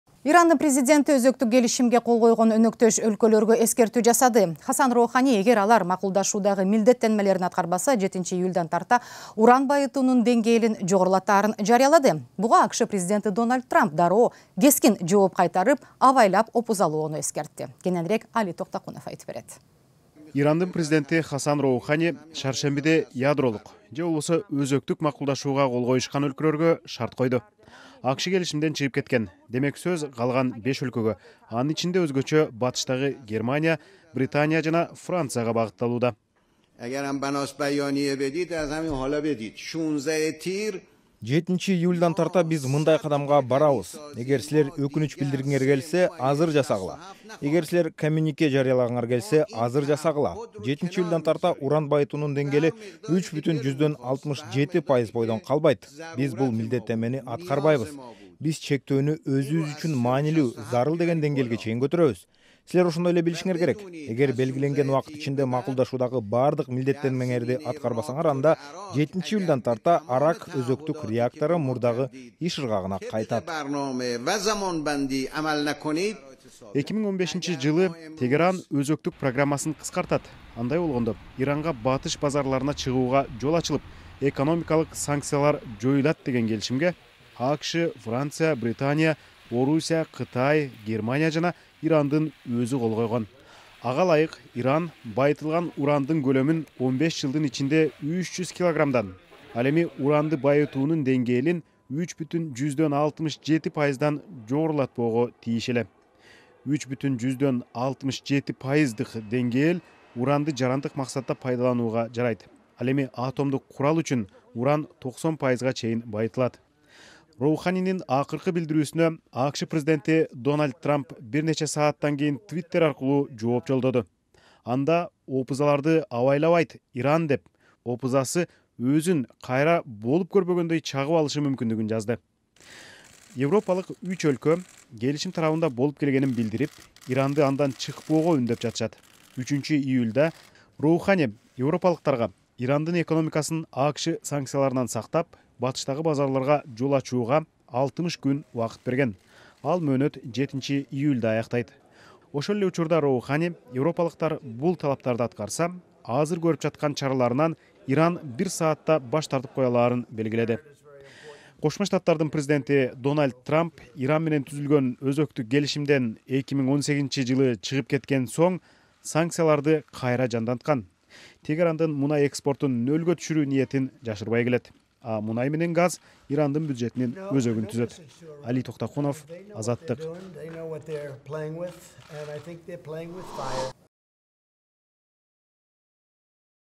Роухани: Иран уранды каалагандай байыта берет
Роухан Вашингтон менен макулдашууга жетишилсе, чечим “бир сааттын ичинде” жокко чыгарыларын белгилеп, эл аралык коомчулукту тынчсызданбоого чакырды. Анын билдирүүсүндө, АКШ Иранга карата санкцияларды жеңилдетпесе, Арак шаарындагы реактордун иши калыбына келтирилет.